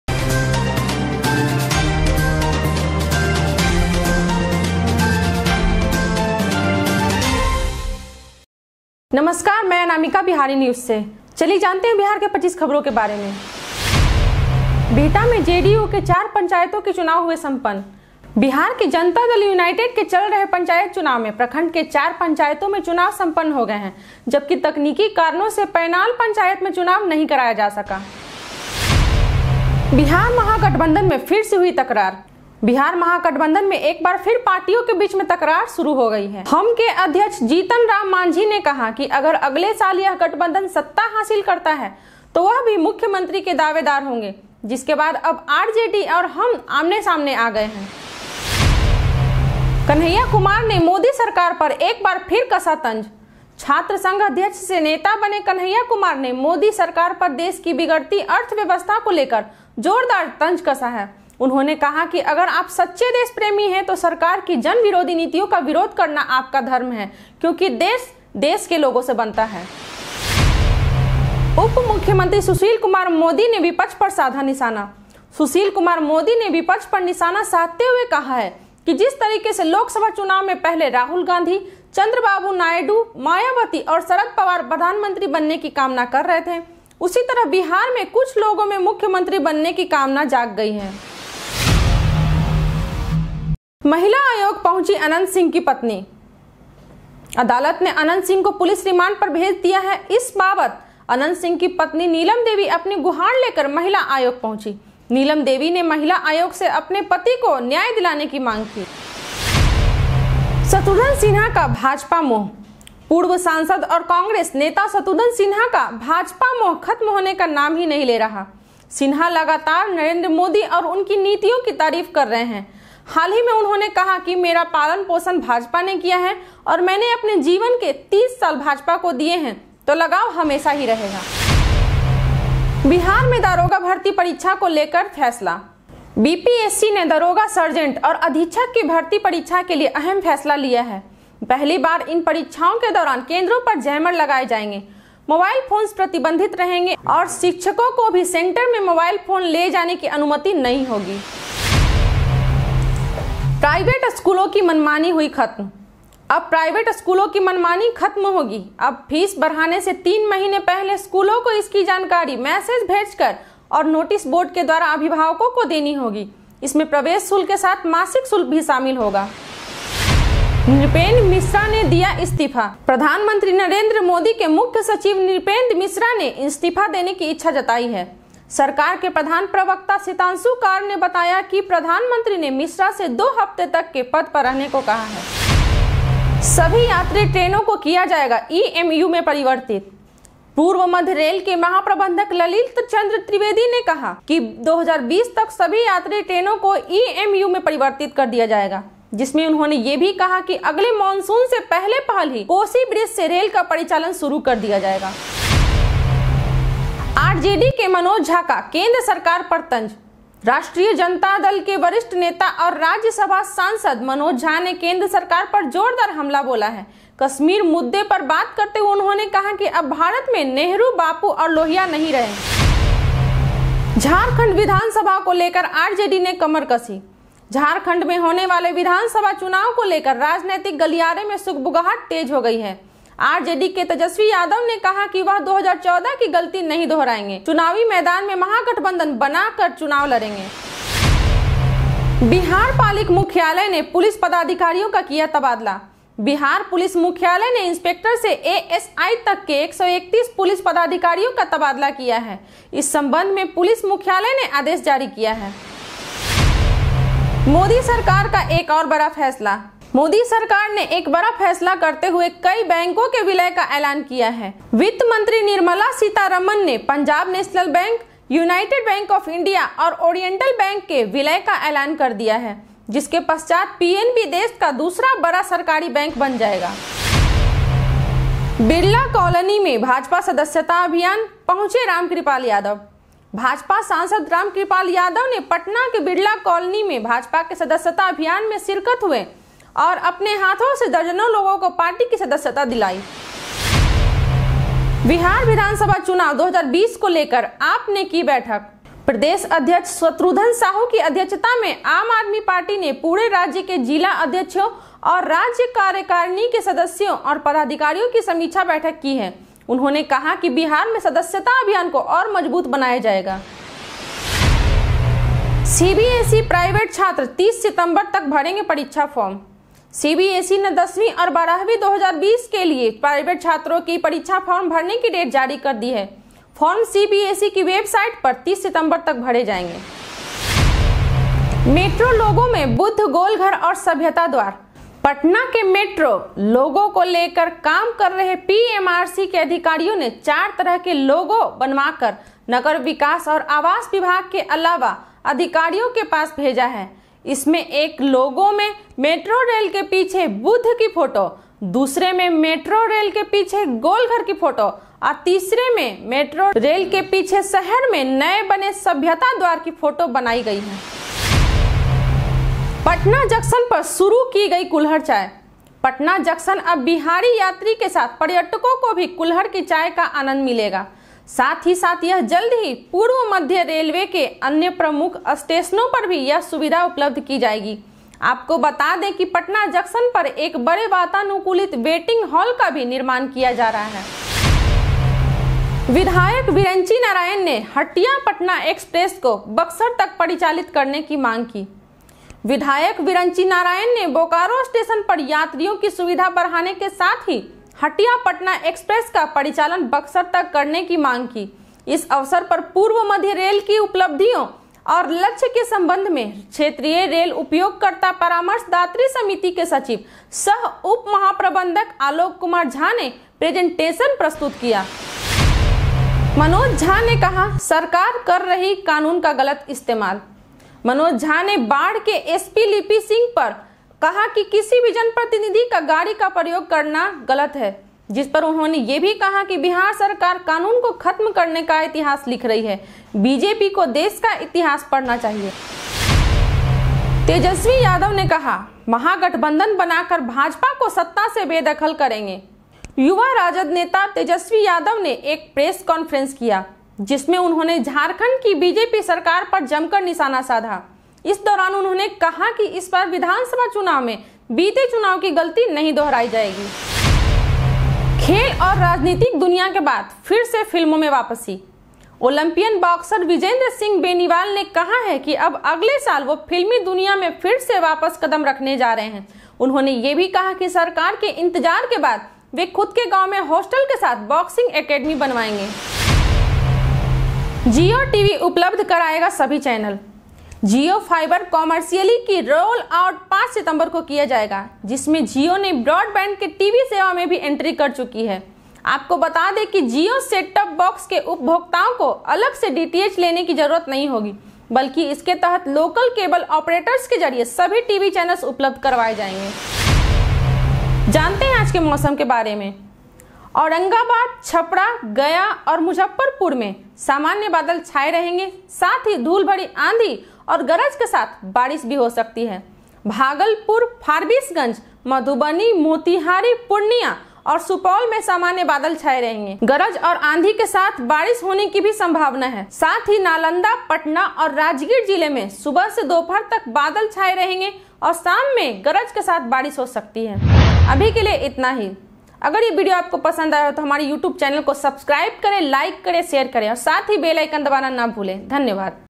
Bihar today latest update from all districts of bihar in hindi from Patna,Gaya and Muzaffarpur.
Get all latest information about districts wise of Bihar in Hindi.

1. Four JDU panchayat elections were held in Bihta
2. Kanhiya Kumar once again criticized Modi government
3. AnantSingh wife reached the Women's Commission
4. Shatrughan Sinha's BJP fascination
5. Decision regarding Daroga Recruitment Examination in Bihar
6. NripendraMishra resigns
7. All passenger trains will be converted into EMU
8. RJD tightens up about Jharkhand assembly
9. Bihar Police Headquarters transferred police officials
10. Another big decision of Modi government
11. BJP membership drive in Birla Colony, Ramkripal Yadav arrives
12. Strict eye on police patrolling team
13. AAP's meeting on Bihar assembly elections 2020
14. CBSE private students will fill the examination form by September 30
15. Buddha Golghar and Civilization Gate in Metro LOGO
16. Kulhad tea started at Patna Junction
17. MLA Viranchi Narayan demanded to operate Hatia-Patna Express to Buxar.
18. Return to film after sports and political world
19. Know about today's weather
20. Jio TV will provide all channels